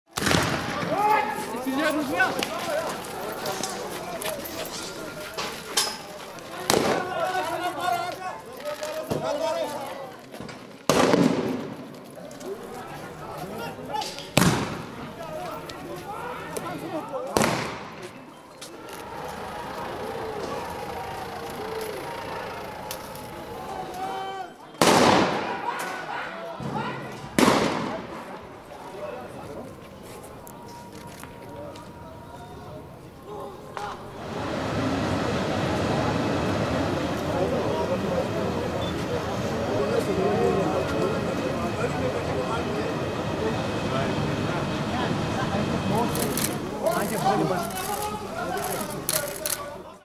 Tote nach Ausschreitungen in Srinagar
Im von Indien verwalteten Teil des auch von Pakistan beanspruchten Himalaya-Tals kommt es immer wieder zu Gewalt. …